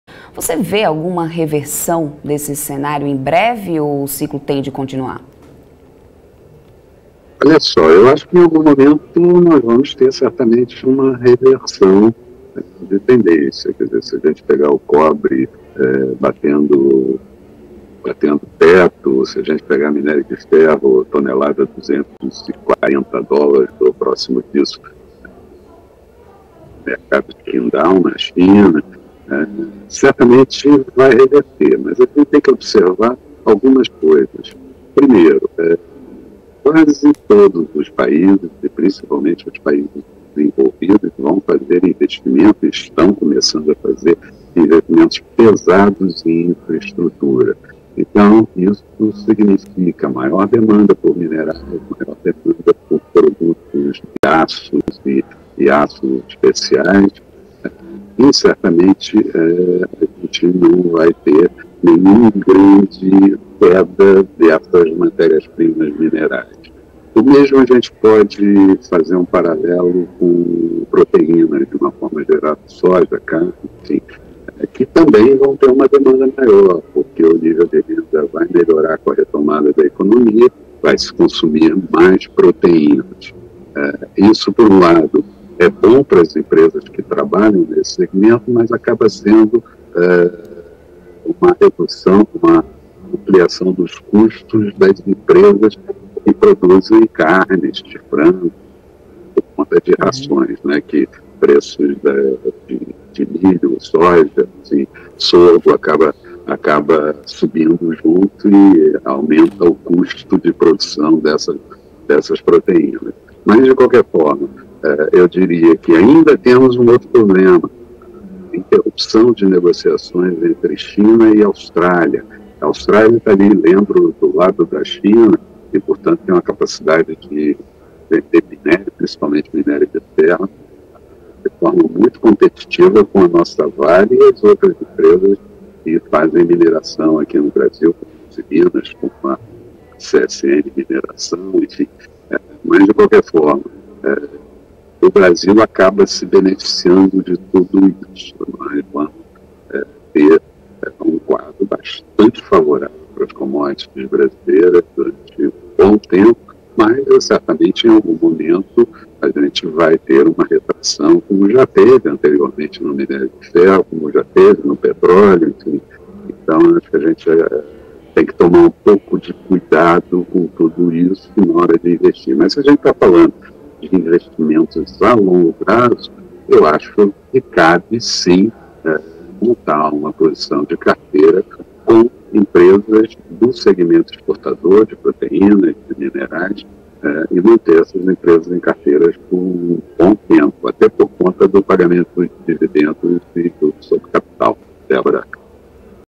Economista da Modalmais avalia reversão de tendência para commodities
Álvaro Bandeira, economista-chefe do Modalmais, avalia cenário de commodities diante de aumento da demanda e preço do minério.